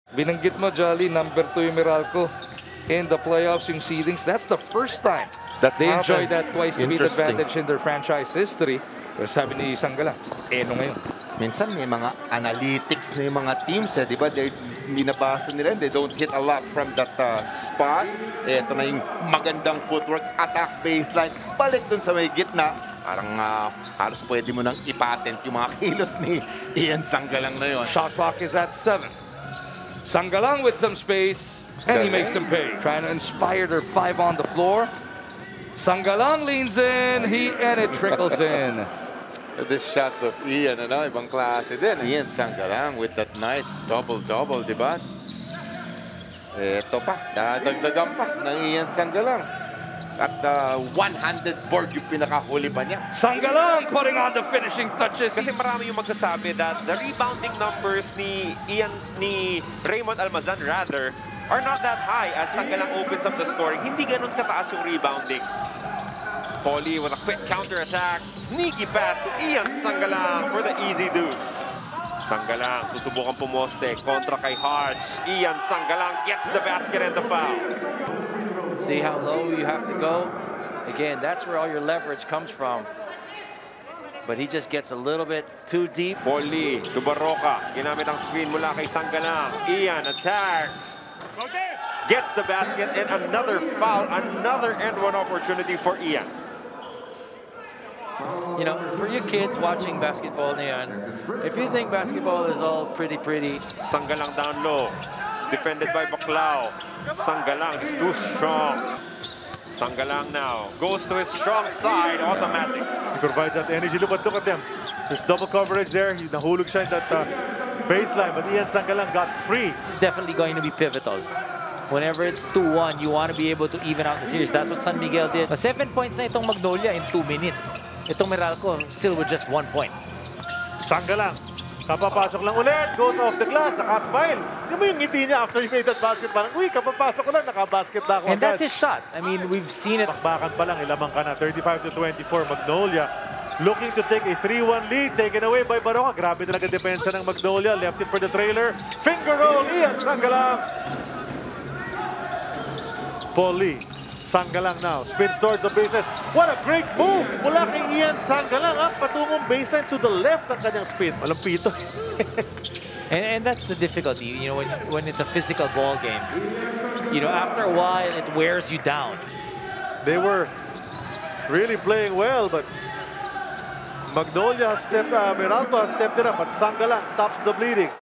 Ian Sangalang - Player of the Week | 2021 PBA Philippine Cup
CONSISTENT IAN! 💪

Major factor sa kasalukuyang 3-1 semis lead ng Magnolia ang steady performance ni Ian Sangalang para maging PBA Player of the Week!

Close to a double-double with 16 points and 9.3 rebounds ang naging average ni Sangalang sa last three games.